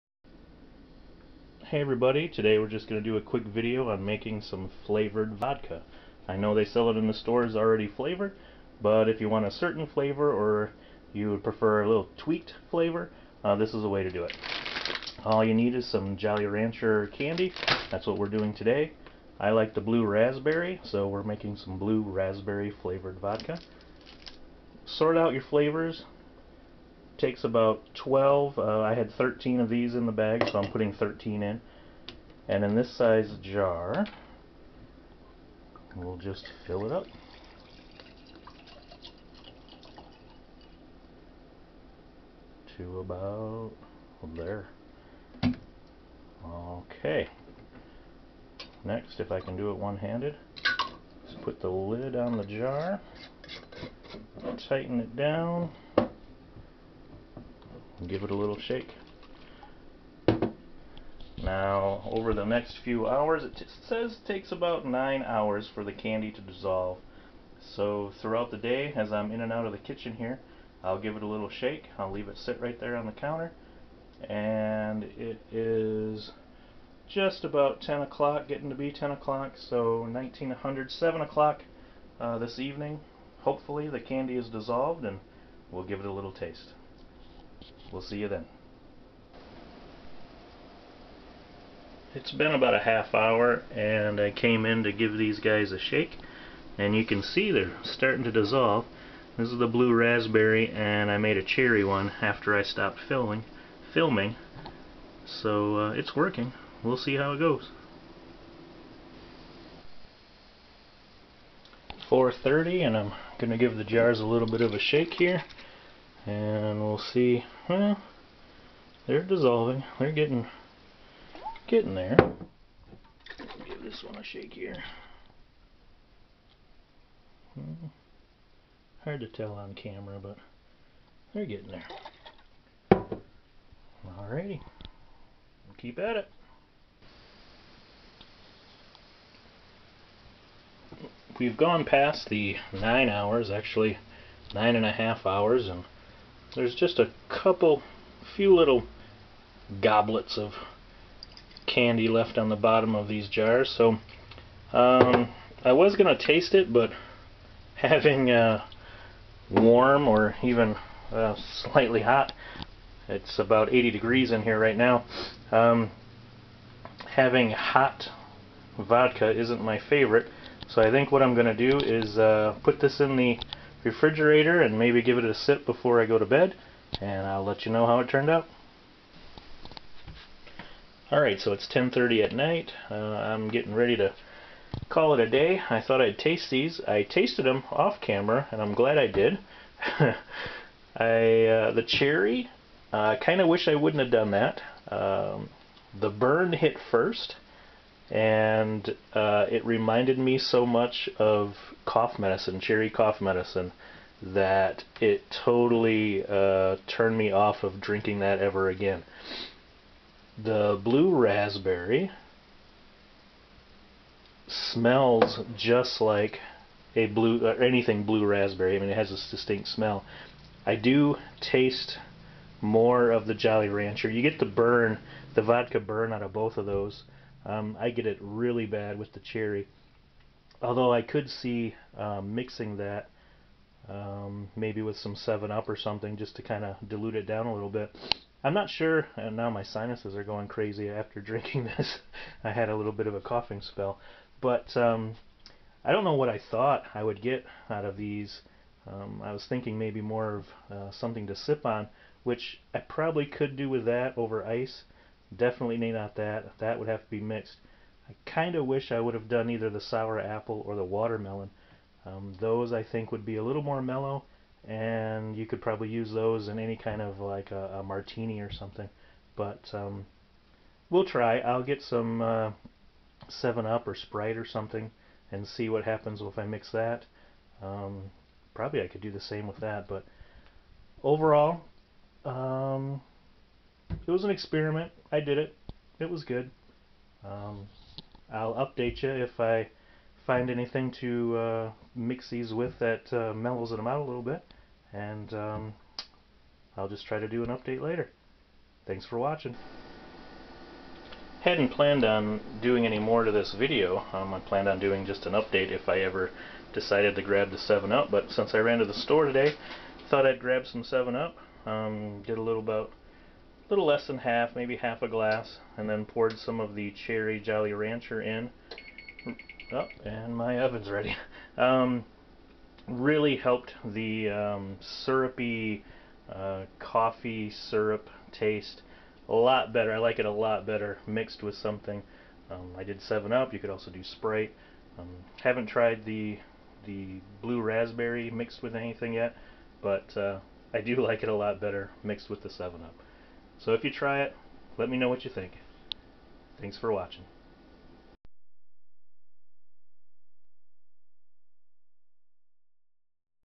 Jolly Rancher Vodka
Love Jolly Ranchers, Don't mind vodka...why not mix them!
My try at mixing the two. Wasn't sure what I thought I would get (maybe a sweet sipping vodka?) but it wasn't what I expected.
Glad I found the 7-Up! The cherry tastes so much better with that!